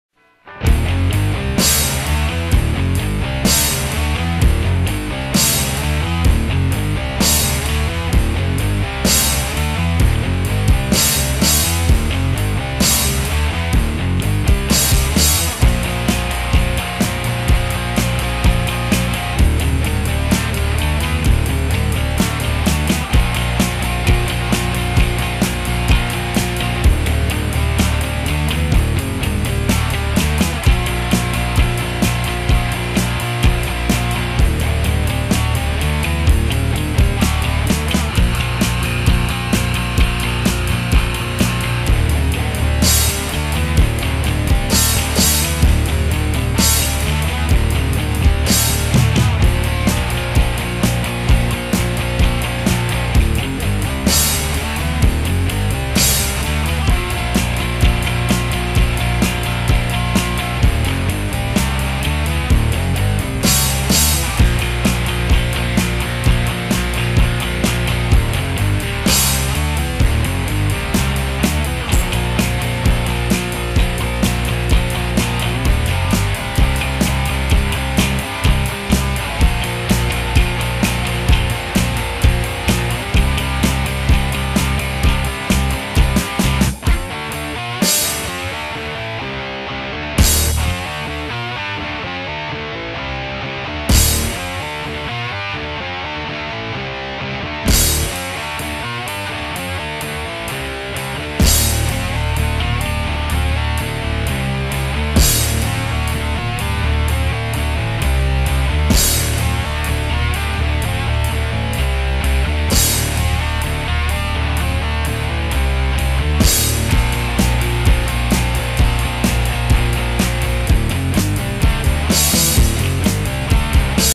Guitar Collection 09'
My collection of guitars put to original music by me.